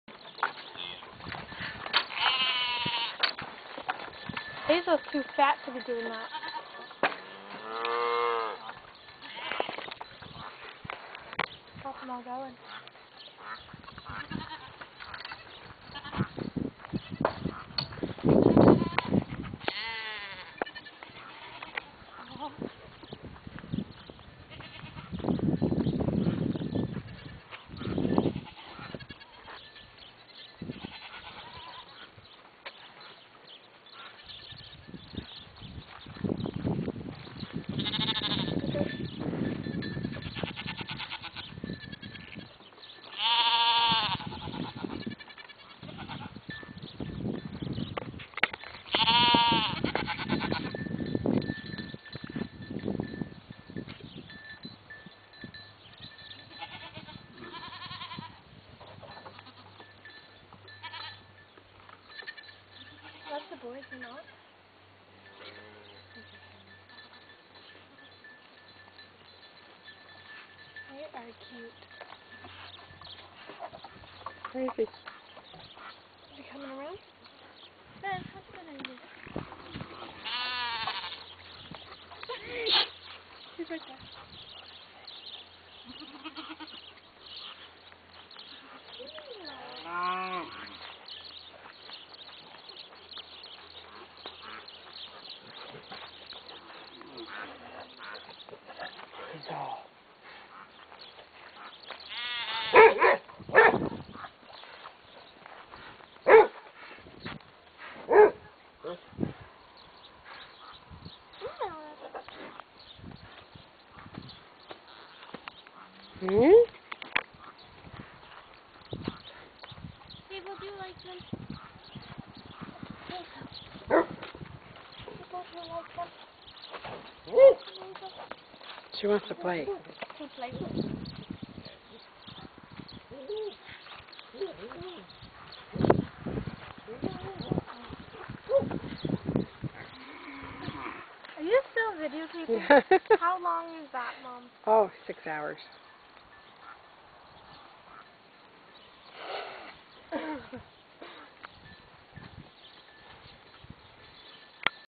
Highland Calves Playing
Highland calves(Hillary and Harry- our bottle babies) first time in the paddock with all the other animals. They sure like to play.  July 29 2011